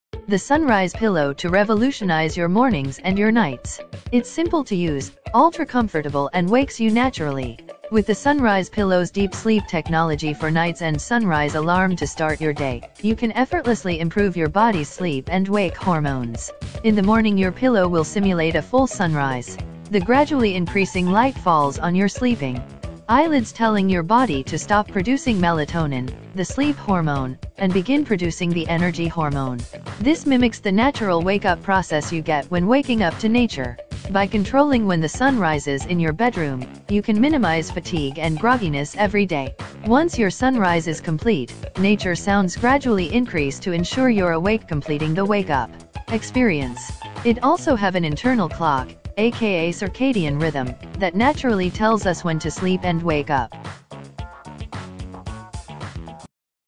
Sunrise Smart Pillow tracks your sleep, wakes you with light, nature sounds
The Sunrise Pillow to revolutionize your mornings and your nights. It’s simple to use, ultra-comfortable and wakes you naturally. With the Sunrise Pillow's Deep Sleep technology for nights & Sunrise Alarm to start your day, you can effortlessly improve your body's sleep & wake hormones.

In the morning your pillow will simulate a full sunrise. The gradually increasing light falls on your sleeping eyelids telling your body to stop producing Melatonin (the sleep hormone) and begin producing the energy hormone. This mimics the natural wake-up process you get when waking up to nature. By controlling when the sun rises in your bedroom, you can minimize fatigue and grogginess every day.

Once your sunrise is complete, nature sounds gradually increase to ensure you're awake--completing the wake-up experience.

It also have an internal clock (aka circadian rhythm) that naturally tells us when to sleep & wake up. The Sunrise Pillow detects your sleep cycle and knows to begin the wake-up process when you're sleeping lightly. Your sunrise will start at the optimal time or up to 15 minutes before your set alarm time-to ensure you wake up feeling fully rested, every day.

Take control of your days by improving the quality of your nights. Your sleep score gives you a better understanding of the most important factor of your day; your sleep. With the Sunrise Pillow app, your sleep trends & history show you how exercise, late nights, and even alcohol consumption affect your score, so you can perfect your sleep.

Waking up to the latest technology is one thing, but falling asleep to it is taking it to the next level. The Sunrise Pillow is equipped to wake you with a smart alarm and sunrise simulation but it can also put you to sleep with 6-deep sleep options that are sure to put you to sleep faster.

Supercharge your mornings or reboot after lunch. Studies show that exposure for 15-30 minutes of blue light can improve productivity, alertness, and also boost your ability to sleep at night. Blue light suppresses melatonin (the sleep hormone) and shifts your circadian rhythm twice as much as regular indoor lighting. Which can benefit you greatly when you want to focus and get something done.

The way we’ve been waking up is unnatural, even harmful and needs to change. Waking from a deep sleep not only sabotages your morning energy & mood but more importantly your daily momentum.
When you start the day off groggy you are more likely to make poor choices that turn into a chain reaction that negatively affects your day, week, and year.